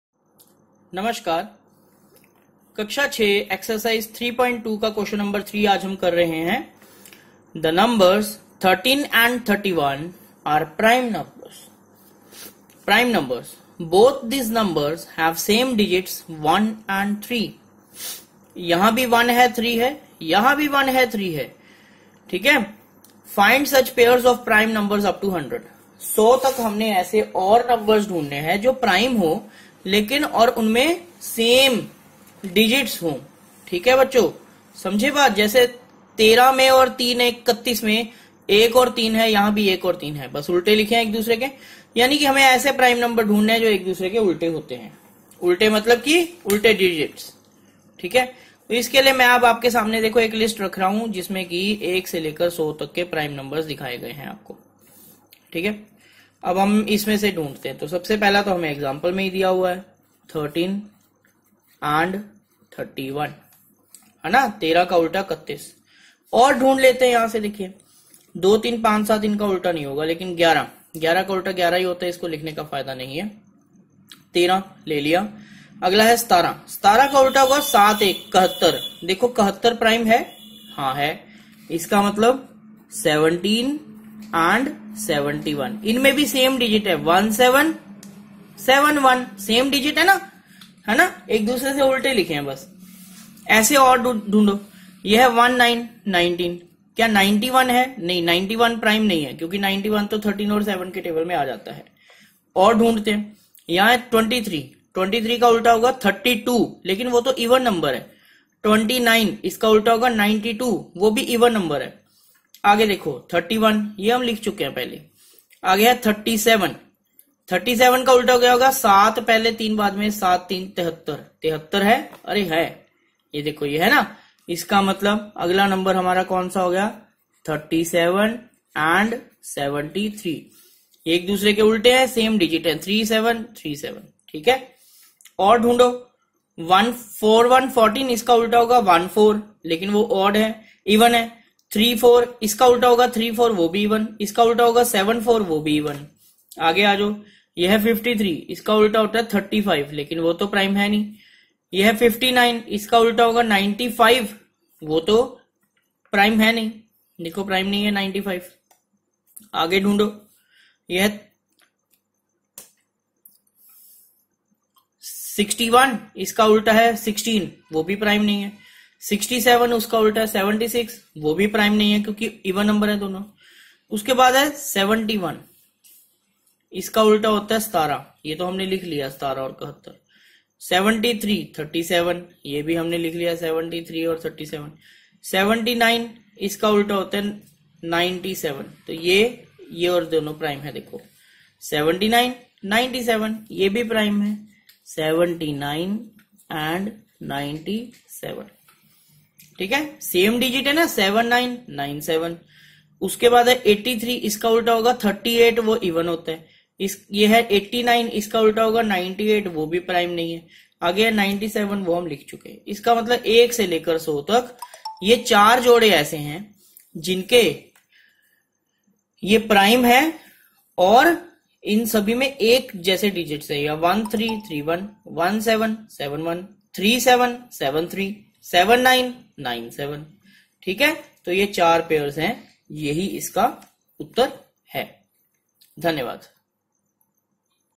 CLASS 6 EX 3.2 Q3 PRIME NUMBERS
This video helps to learn about CLASS 6 EX 3.2 Q3 PRIME NUMBERS in an easy, playful and interesting manner.